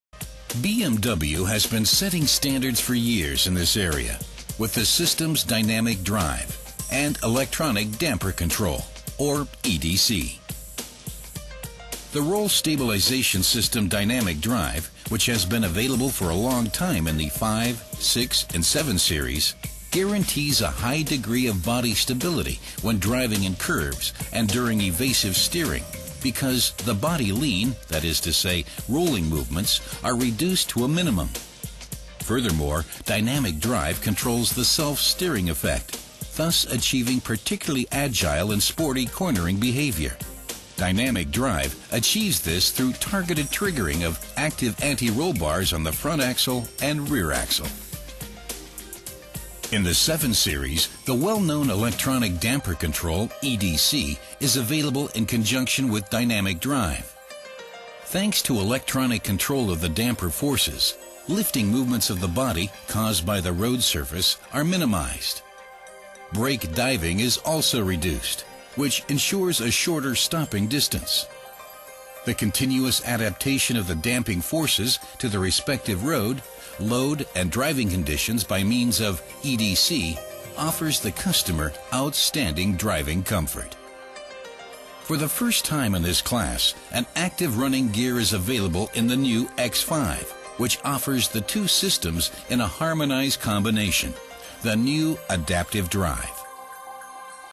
BMW X5 Adaptive drive (2)
Op de nieuwe X5 introduceert BMW een Adaptive Drive systeem naar het voorbeeld van de 7-serie. Het omvat verstelbare stabilisatoren en variabele schokdemping, waarmee de carrosserie tijdens het rijden zoveel mogelijk horizontaal wordt gehouden. In een aantal films wordt Adaptive Drive uitvoerig toegelicht, waar het voor dient en hoe het werkt.